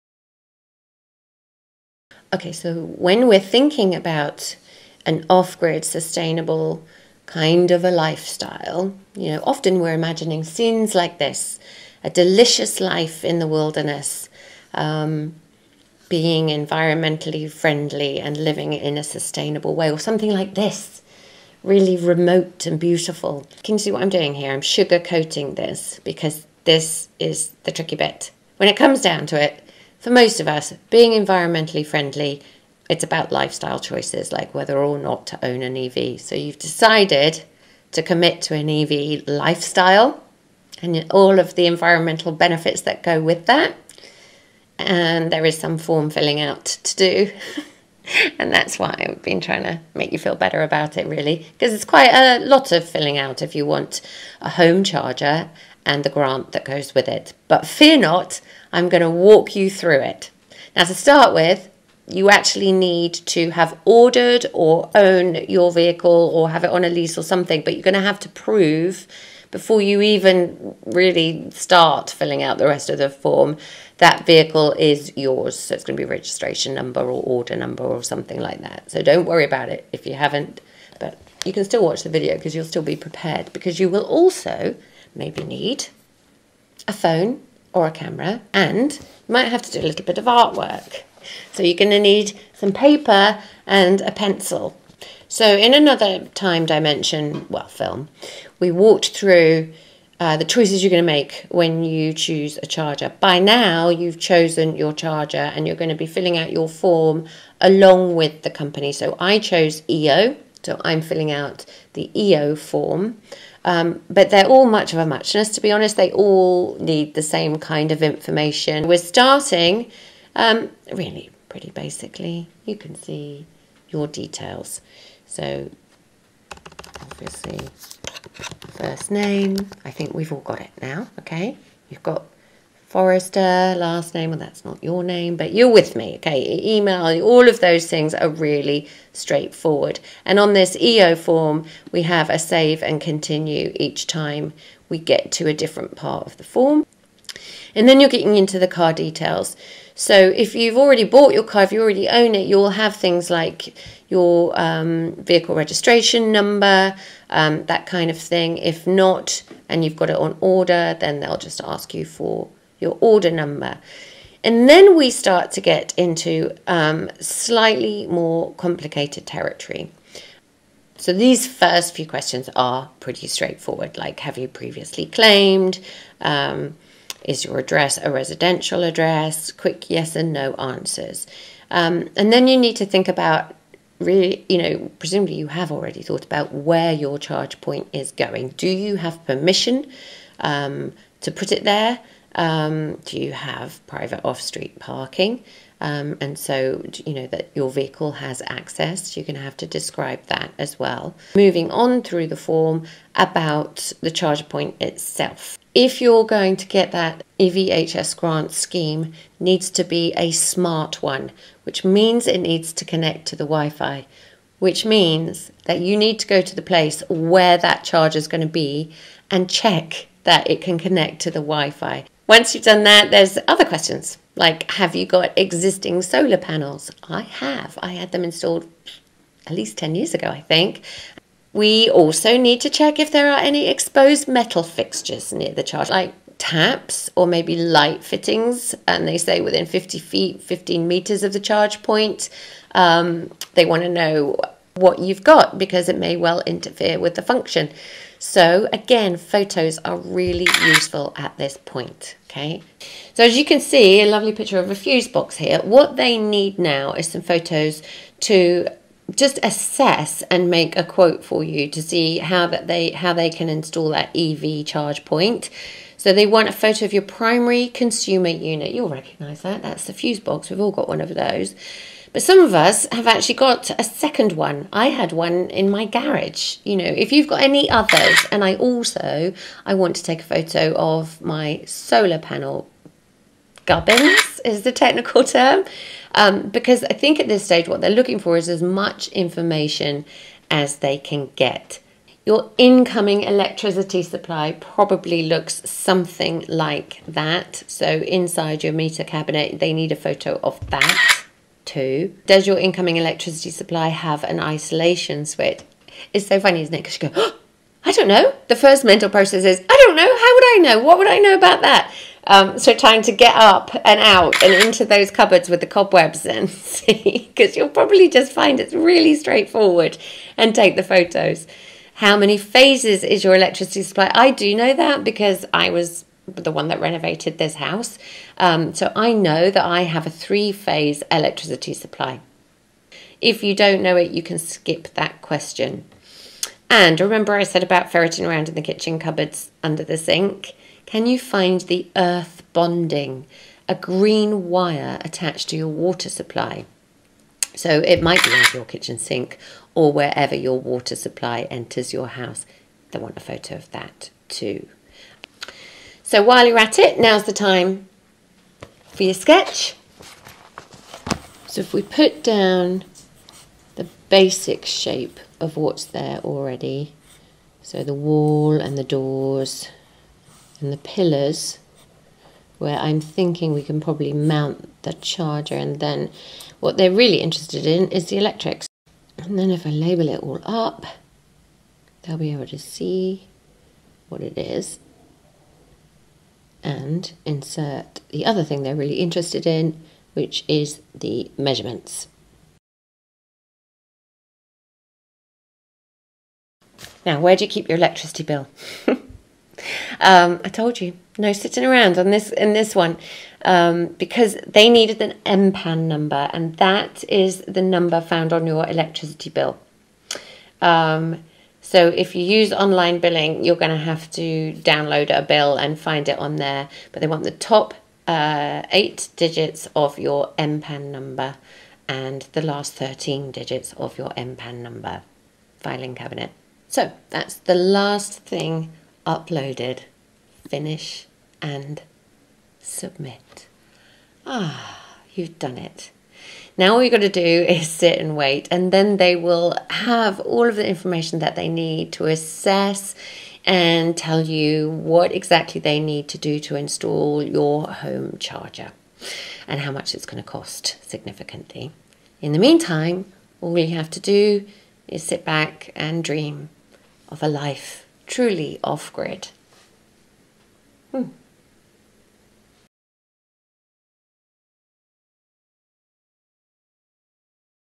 Step by step guide to form filling for an EV home charger
Time to get to down to the nitty gritty! It's 10 minutes of your life you'll never get back, but it's so worth it to go electric!

A step by step guide to filling in the online form for your home charger. Mine is via EO, but they all need similar information. You'll need a camera (a phone camera is fine), a pen and paper and a tape measure!

I'm also on Twitter, Instagram and Facebook too! find out more